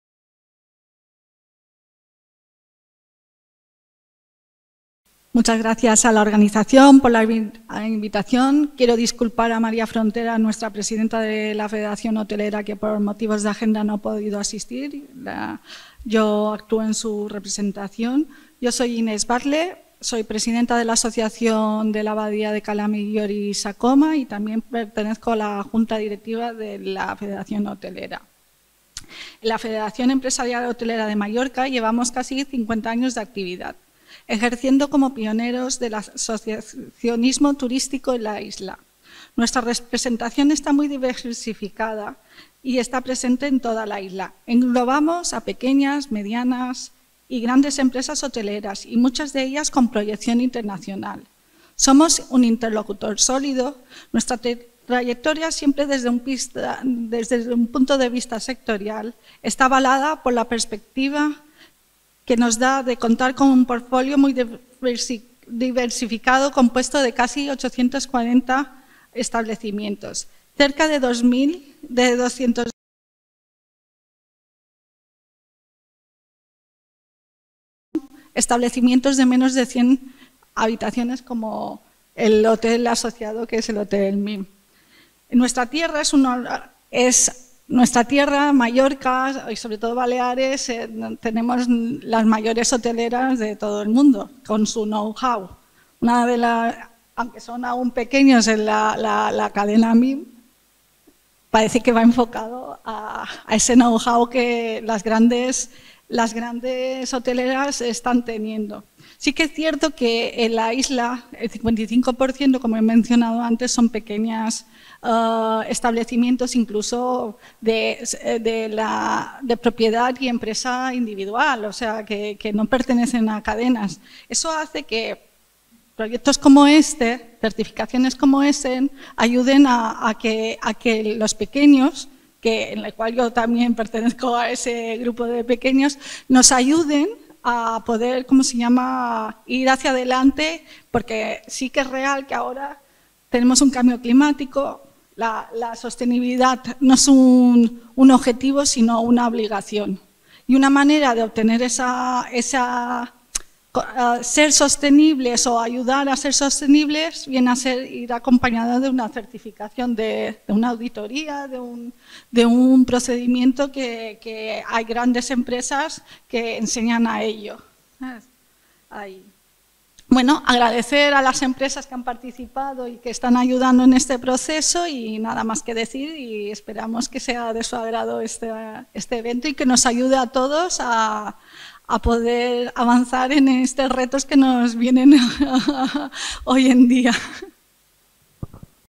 FEHM, Federación Empresarial Hotelera de Mallorca
María Inés Batle Eriksson, Secretaria de la Junta Directiva de la FEHM, Federación Empresarial Hotelera de Mallorca.

Jornada “La sostenibilidad y la salud en el Sector Hotelero”
Caso MiMMALLORCAHotel, primer Hotel certificado LEED V4 BD+C: HOSPITALITY en España.